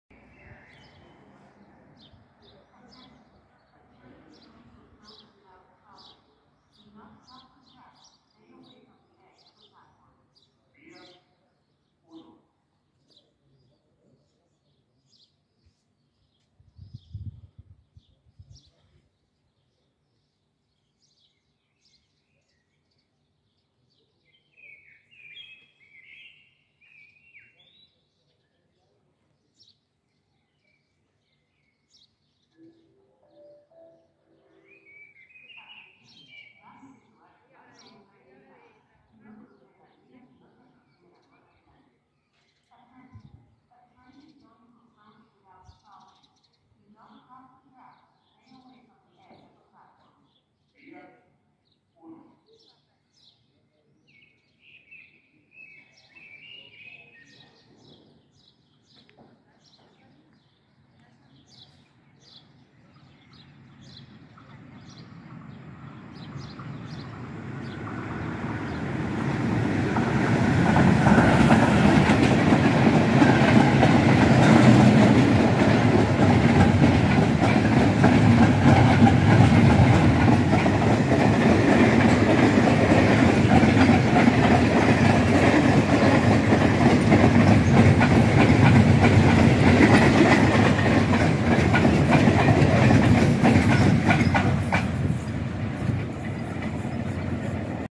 253-029 con teco de boluda cazado por pinto procedente de Sevilla
video realizado el día 14-5-22 a las 15:42 minutos